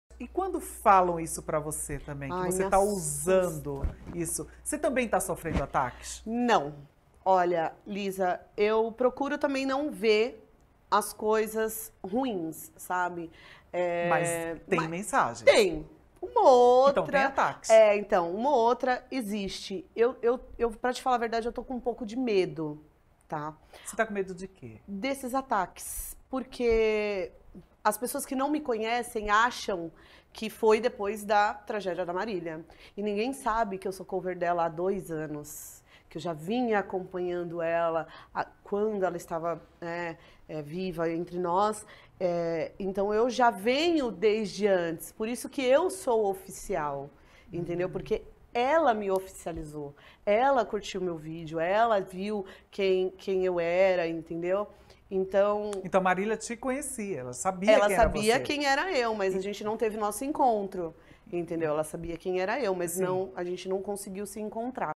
"Tô com medo dos ataques", desabafa Lorena Alexandre, cover da Marília Mendonça
Seja bem-vindo!